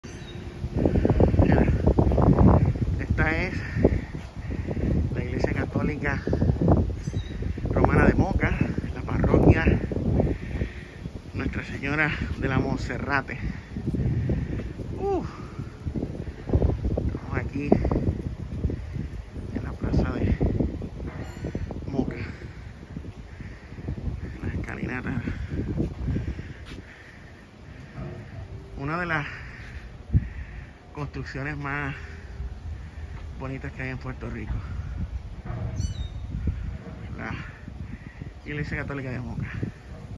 Parroquia católica de Moca
La Parroquia Nuestra Señora de la Monserrate de la iglesia católica romana de Moca, Puerto Rico.